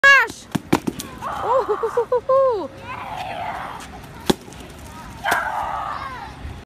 CrossFit Wando Kids Pumpkin Smash 2015
A yearly tradition at Foundation FIT, the CrossFit Wando Kids get together to have a smashing good time.